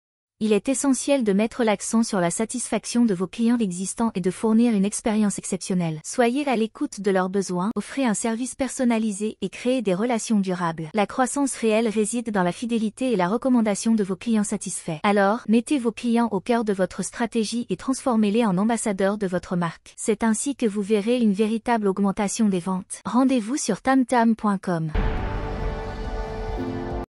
Ne confondez pas followers et clients ----- TamTam Digital School
Ne confondez pas followers et clients. Pour booster vos ventes, misez sur vos clients ! Offrez une expérience unique, fidélisez-les et transformez-les en ambassadeurs de votre marque. Le succès réside dans la satisfaction de vos clients. 💼👥💪